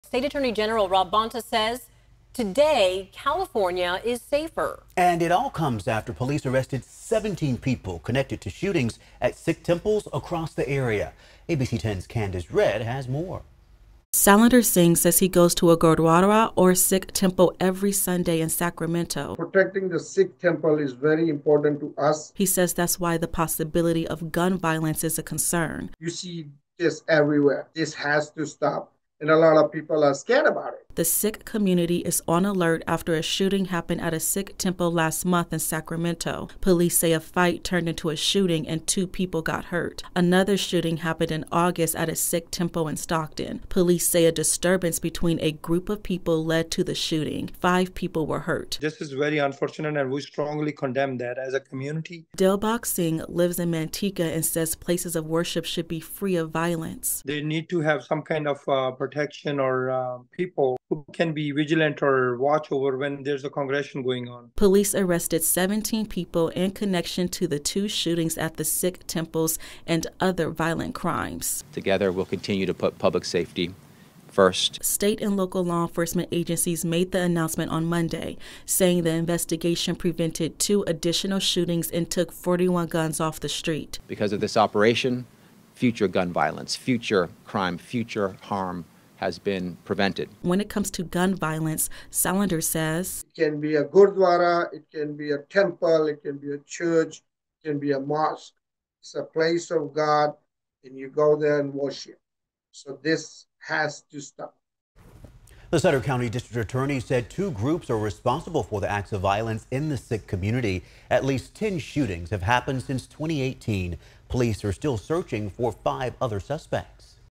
17 arrested after investigation into rival criminal syndicates in Northern California
17 arrested after investigation into rival criminal syndicates in Northern California

Investigators say the two groups are linked to shootings at Sikh temples, and five more people are still wanted.